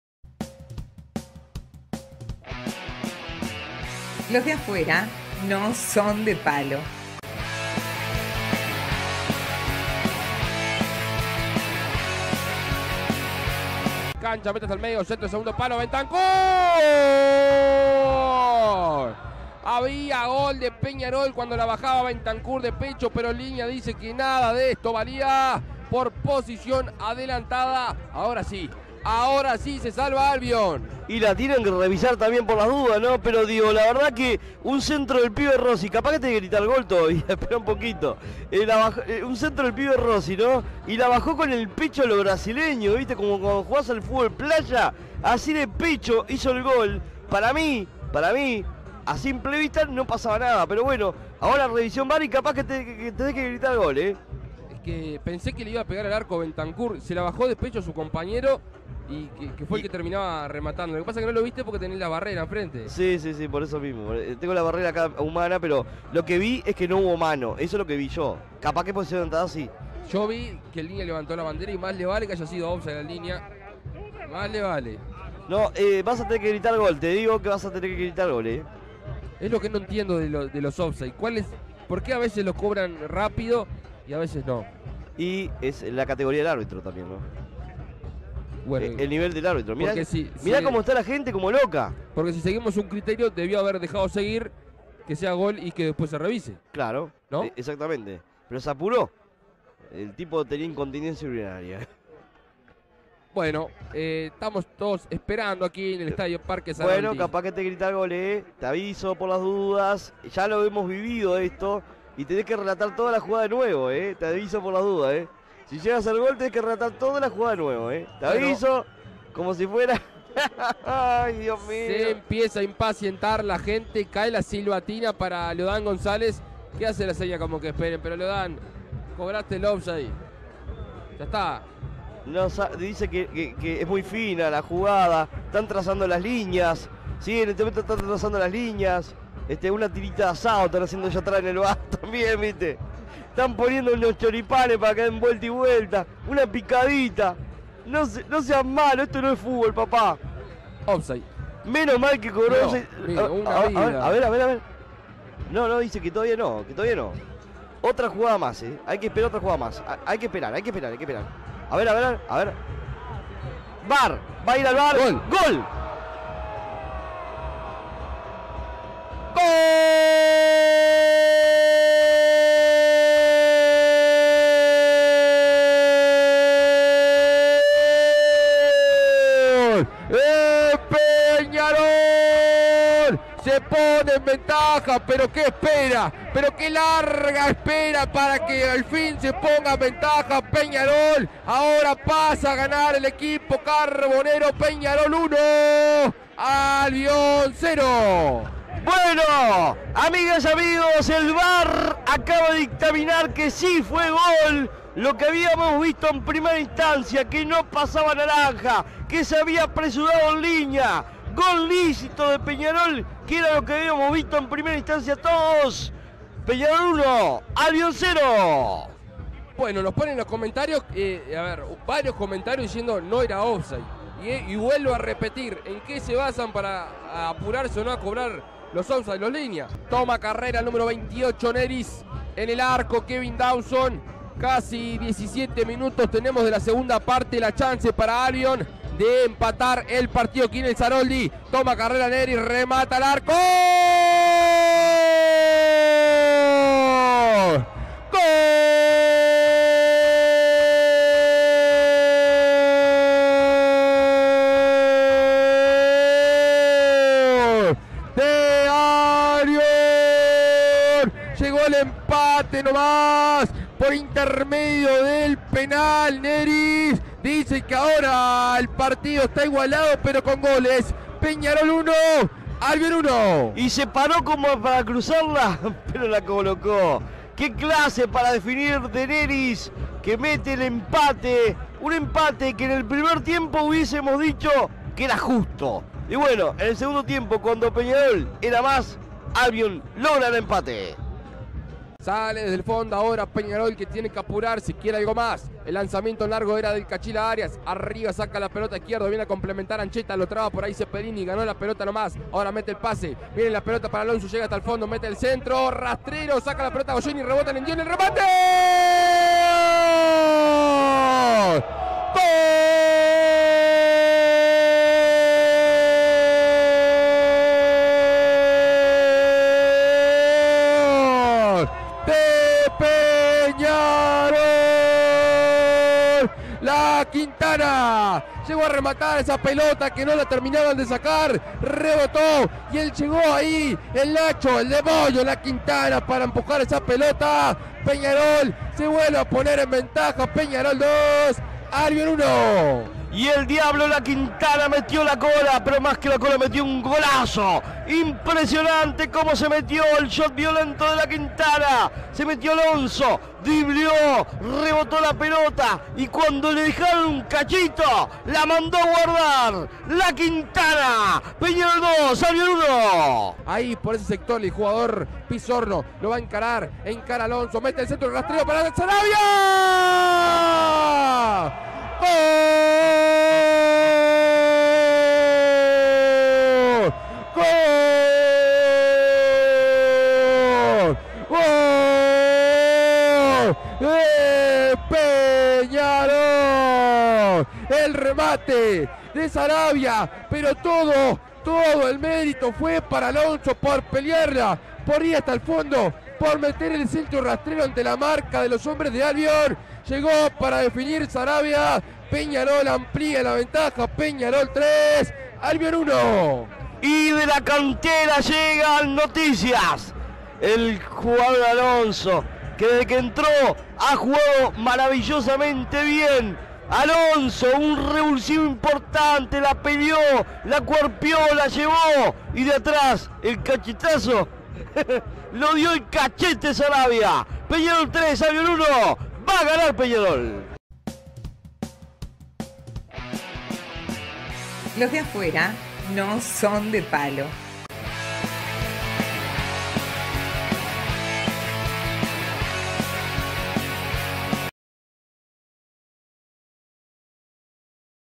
Albion 1 Peñarol 3 - Intermedio 2022 (por Marcos González - LDANSDP)
Los goles del partido, por Marcos González y Gustavo Rodríguez (agradecimiento especial) de @losdeafueranosondepalo.